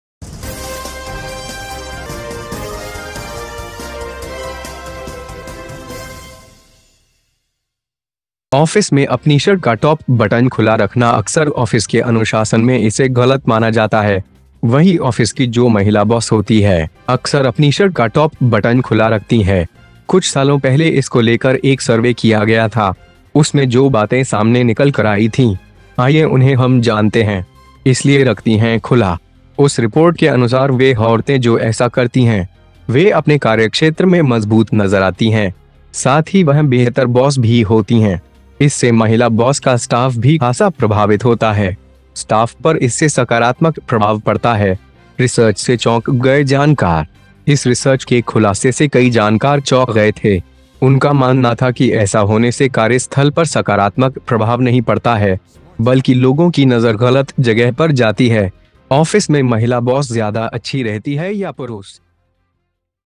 तो इसलिए ऑफिस में महिला बॉस अपनी शर्ट का टॉप बटन खुला रखती हैं जानकार भी चौंक गए
Keeping your shirt top button open in the office is often considered incorrect in office discipline. The female boss of the same office, often holds the top button of its shirt open. A survey was conducted a few years ago. The things that came out in front of them were revealed. Let us know them
Therefore holds open

According to that report the women who do this. They look strong in their work area. They are also better bosses too. This also affects the female boss staff too. This has a positive effect on the staff.

Shocked researcher

Many researchers from this research revealed the chowk. He believed that this does not have a positive effect on the workplace. Rather people's eyes go to the wrong place. Female boss is better or better in the office